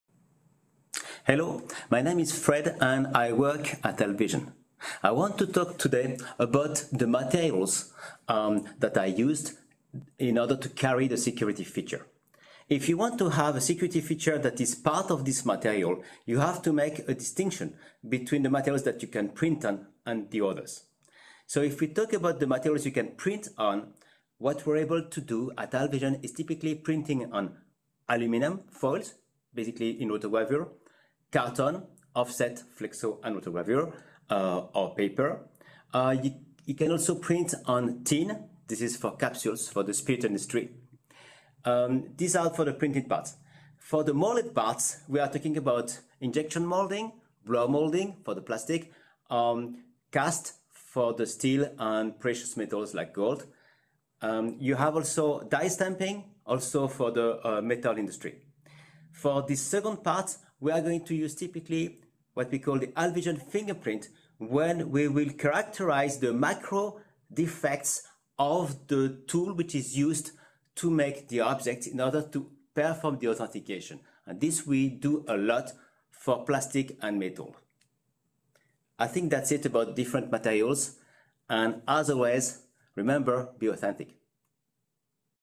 Which materials can be used to carry the security features?
They can be separated into two distinct categories:
- Printed materials
- Other materials

Which printed materials are compatible with AlpVision?
- Aluminum foil
- Folding boxes
- Wine and spirits thin capsules
- Plastic bags

Basically, AlpVision anti-counterfeiting solutions like Cryptoglyph can be applied to any material that can be used in offset, flexography, or rotogravure printing.

Regarding other types of products, AlpVision technologies are particularly well suited for
- Molded products
      - Injection Molding
      - Blow molding
- Cast products (such as gold ingots)
- Die-stamped products (such as minted ingots or other precious products)

AlpVision Fingerprint technology is best suited for these kinds of projects, as it takes into account the intrinsic characteristics of the products that come from the mold.